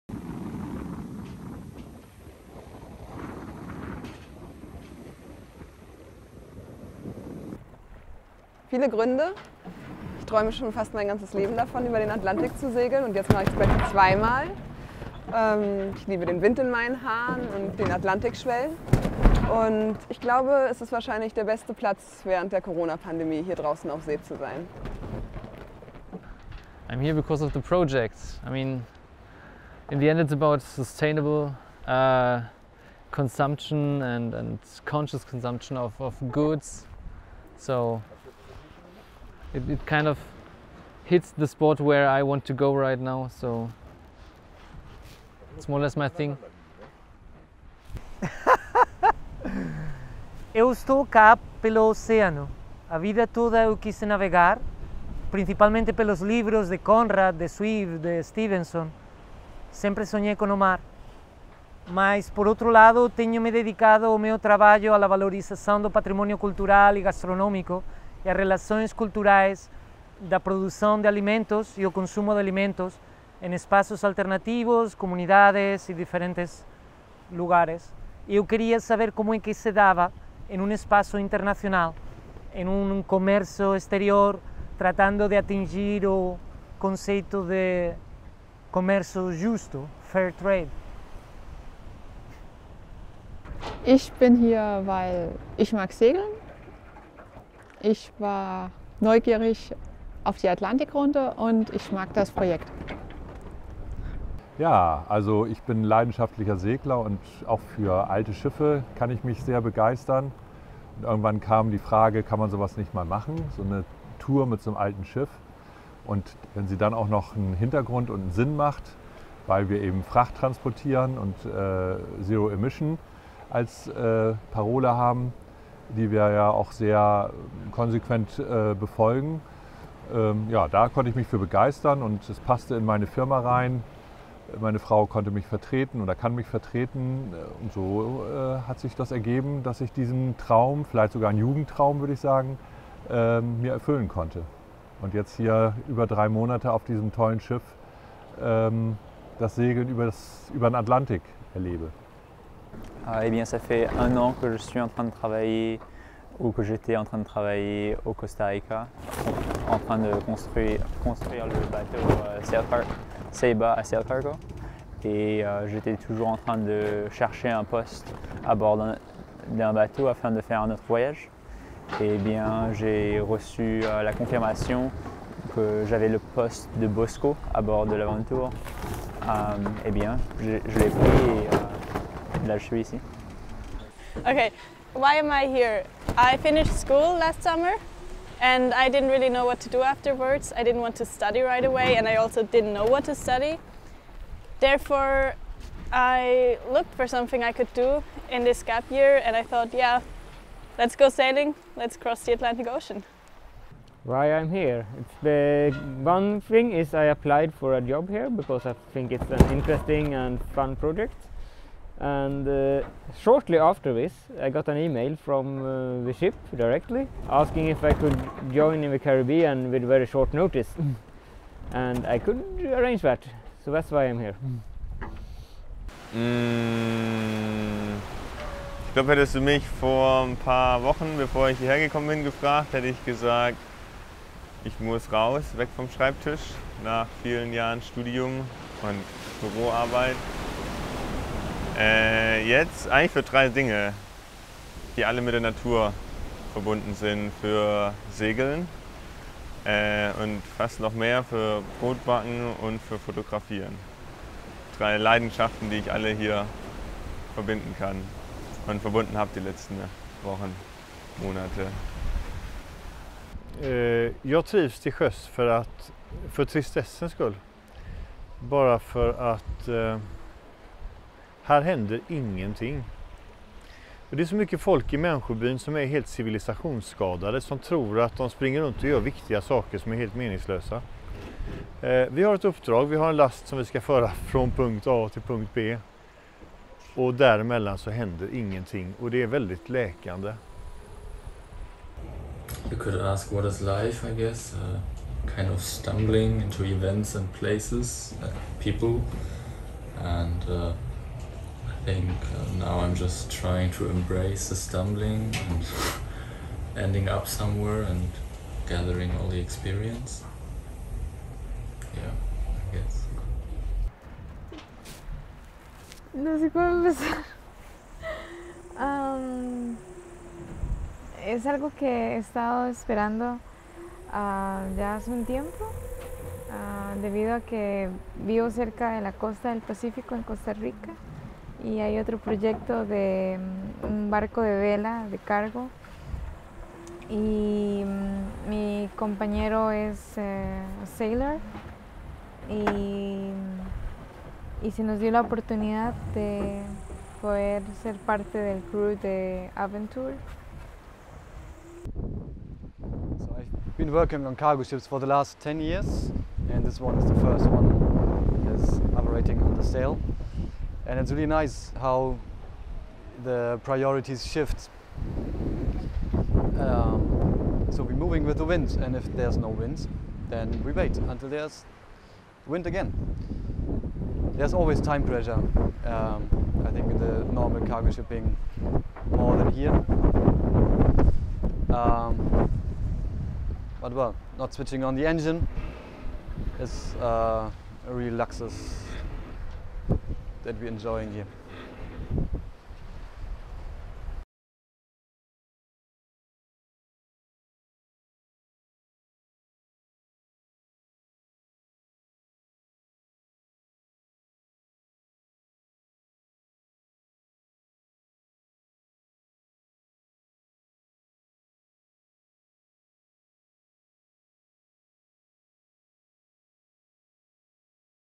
What brings you aboard the AVONTUUR?
A film by our crew members Dan and Chris. Learn more about life on board and the motivation of our shipmates on Voyage 8 in short interviews live from our cargo sailing vessel on the Atlantic Ocean.